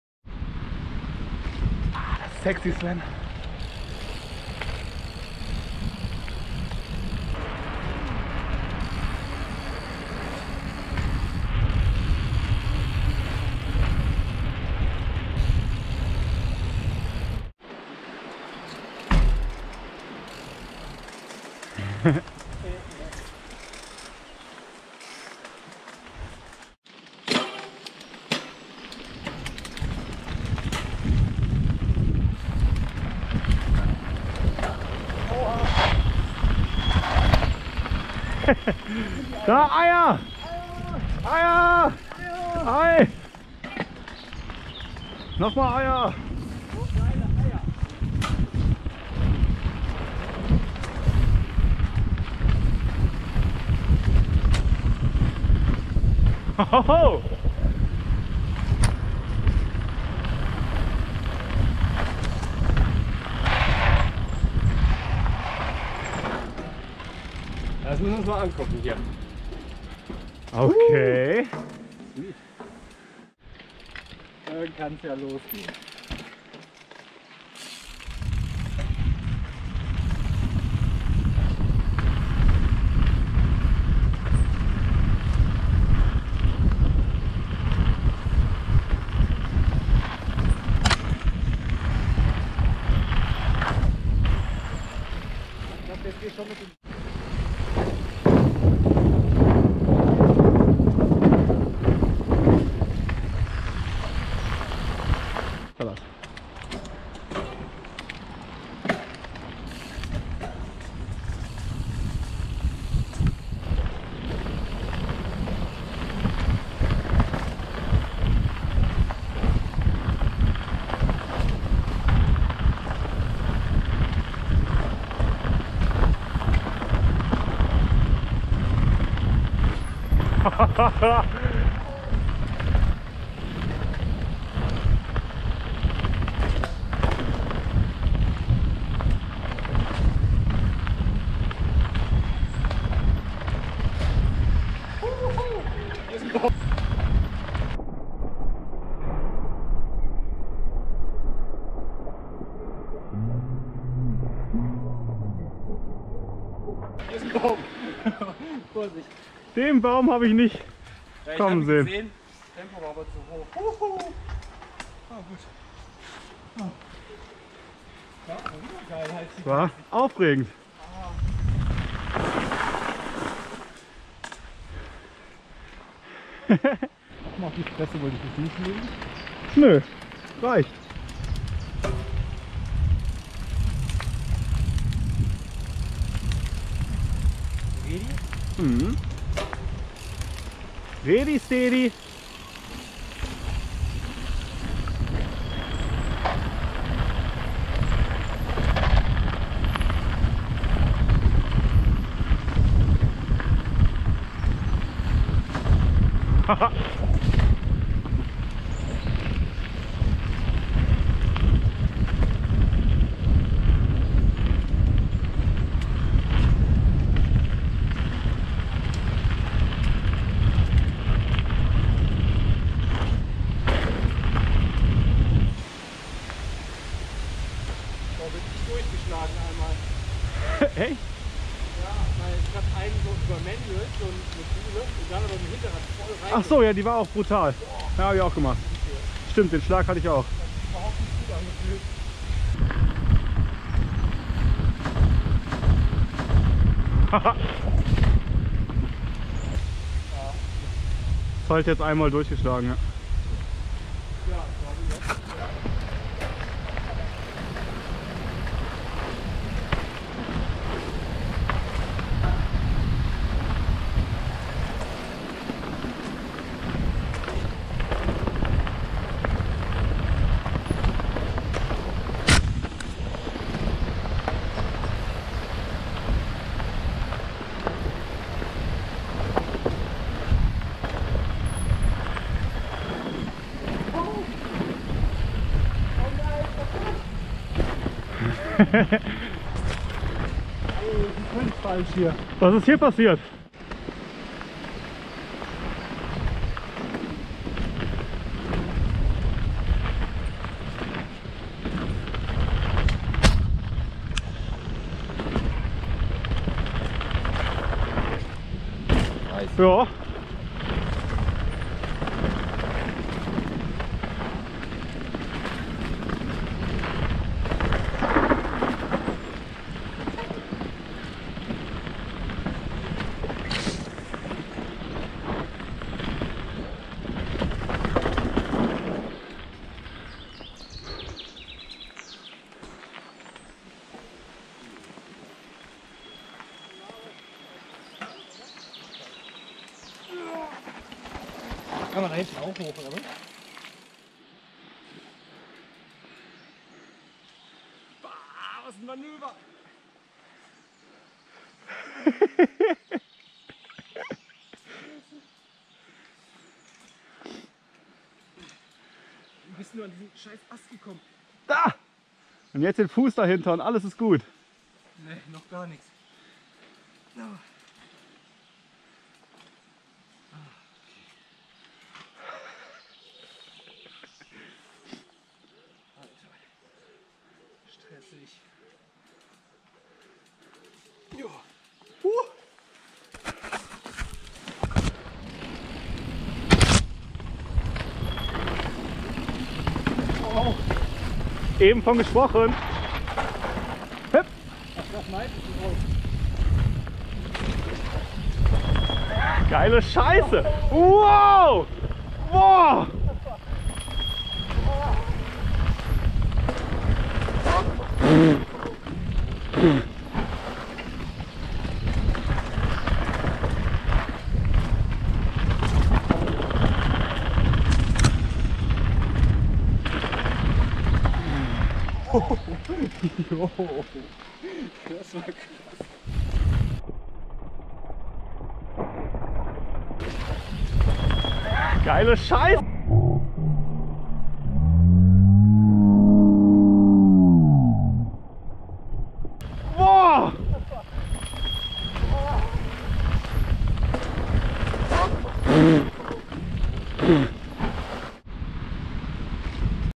Trailcheck Würselen
Planlose MTB Enduro Tour von Würselen aus in die umliegenden Wälder inkl. überraschend kindischem Geschrei meinerseits